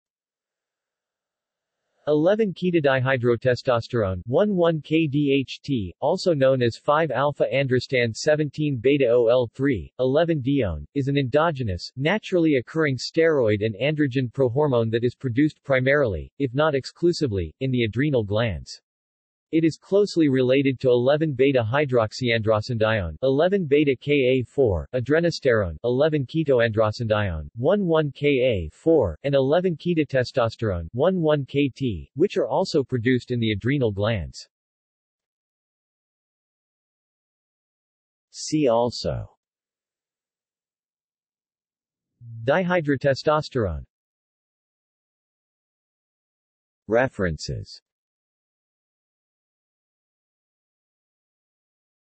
11-Ketodihydrotestosterone | Wikipedia audio article
This is an audio version of the Wikipedia Article:
11-Ketodihydrotestosterone

SUMMARY
11-Ketodihydrotestosterone (11-KDHT), also known as 5α-androstan-17β-ol-3,11-dione, is an endogenous, naturally occurring steroid and androgen prohormone that is produced primarily, if not exclusively, in the adrenal glands. It is closely related to 11β-hydroxyandrostenedione (11β-KA4), adrenosterone (11-ketoandrostenedione; 11-KA4), and 11-ketotestosterone (11-KT), which are also produced in the adrenal glands.